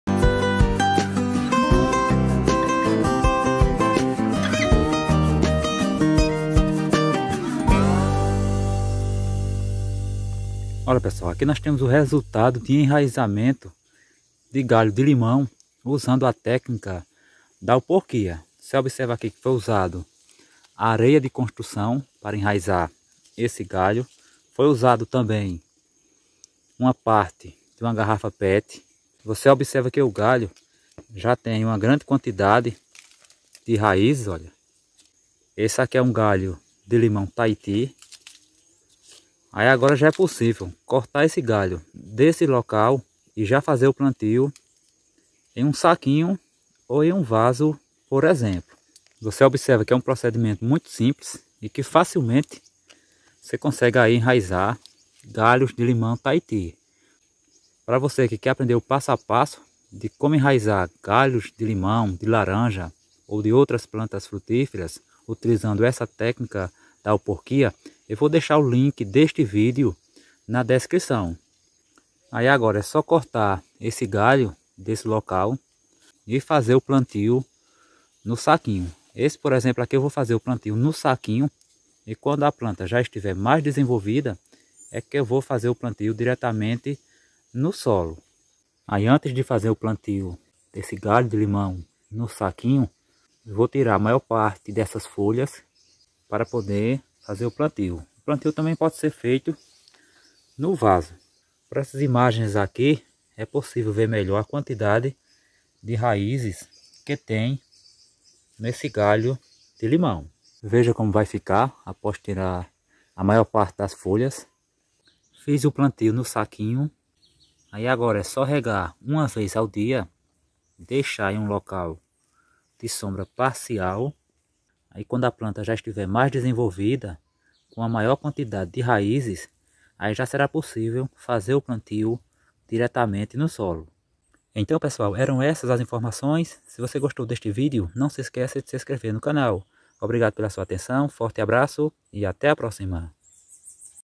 COMO ENRAIZAR LIMÃO TAITI
Nesse vídeo mostro como enraizar limão taiti, falo de uma técnica muito simples para plantar limão  pelo galho e ter bons resultados de enraizamento. Com isso você vai aprender como fazer como fazer muda de limão taiti.